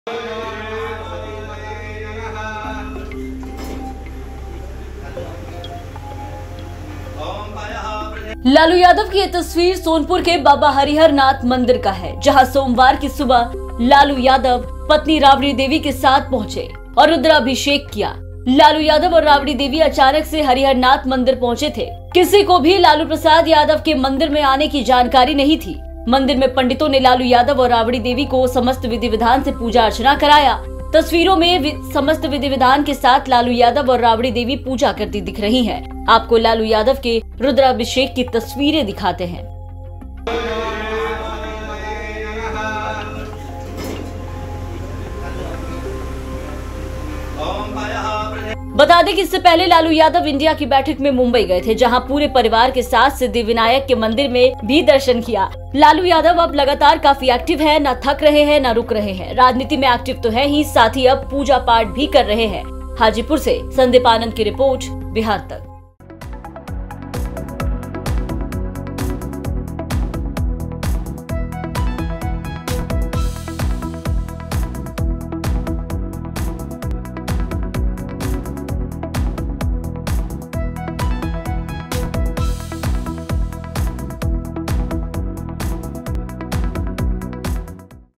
Lalu Yadav, Rabri Devi अचानक पहुंचे Hariharnath Temple, Lord Shiva का किया रुद्राभिषेक | Bihar Tak
लालू यादव की यह तस्वीर सोनपुर के बाबा हरिहरनाथ मंदिर का है.. जहां सोमवार की सुबह लालू यादव पत्नी राबड़ी देवी के साथ पहुंचे और रुद्राभिषेक किया..  लालू यादव और राबड़ी देवी अचानक से हरिहरनाथ मंदिर पहुंचे थे..